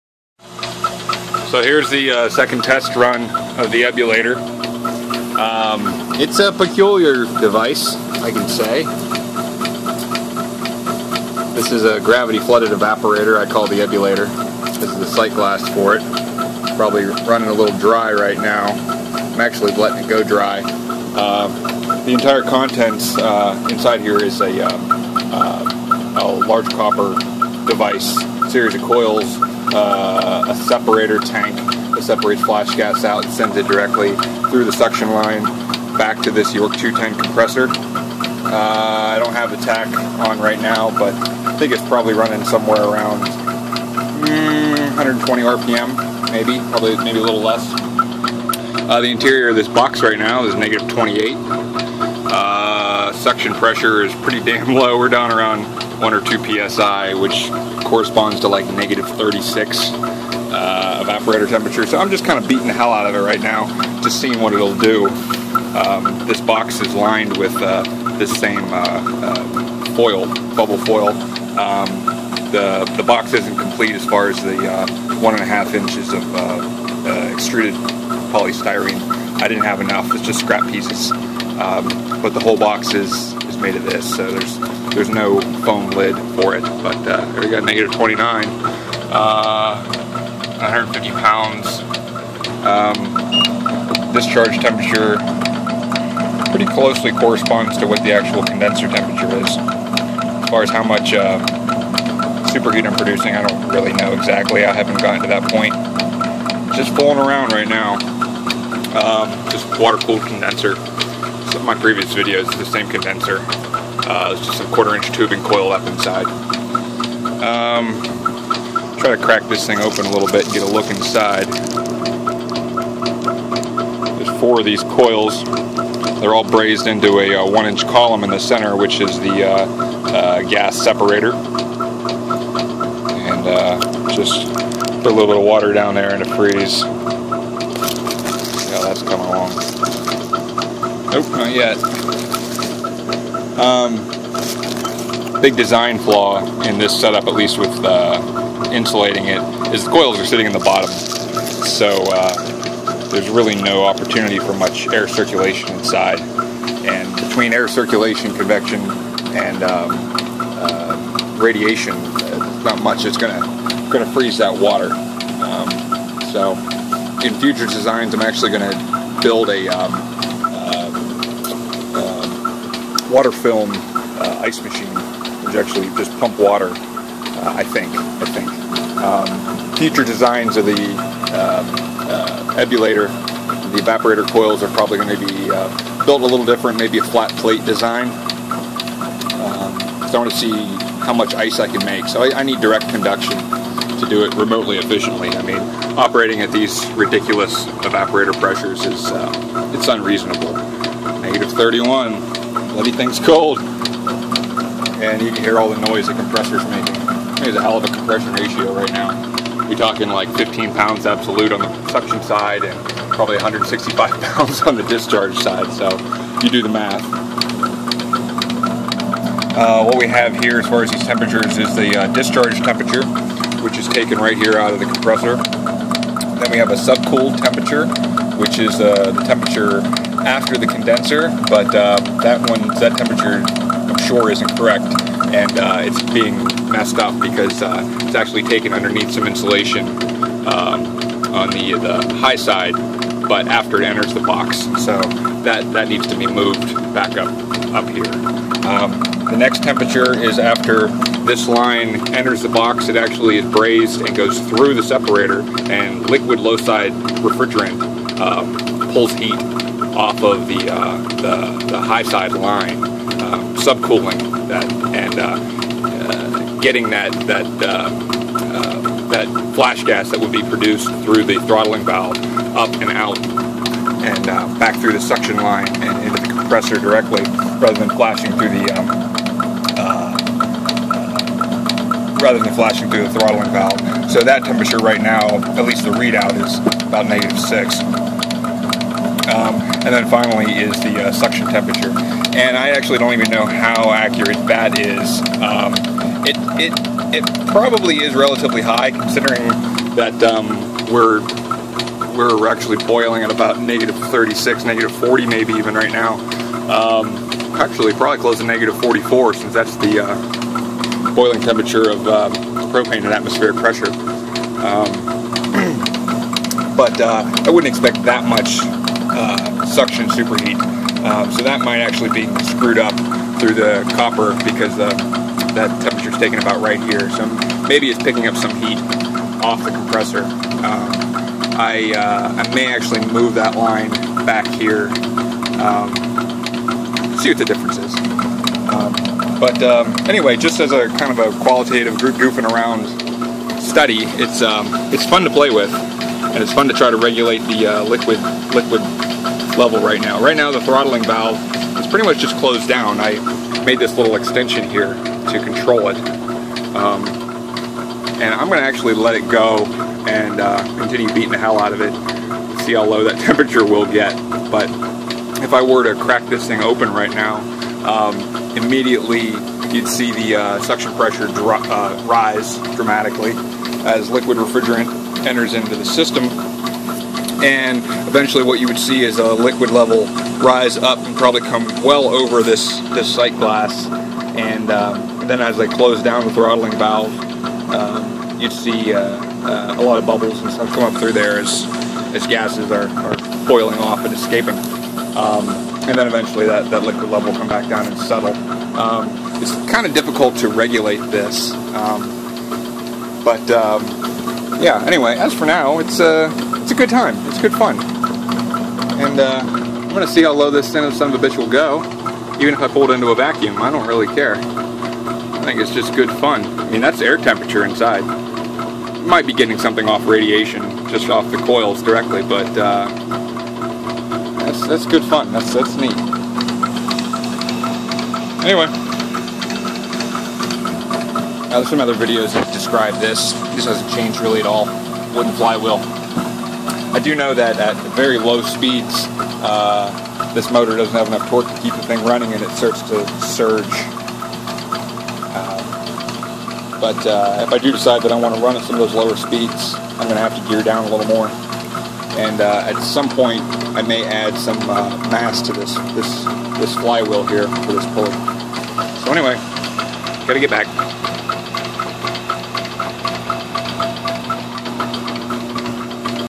Ebullator - Second Run
Beating the hell out of the refrigeration system.  Ran the suction pressure down to a partial vacuum, and had the inside of the insulated box down to -36 degrees Fahrenheit.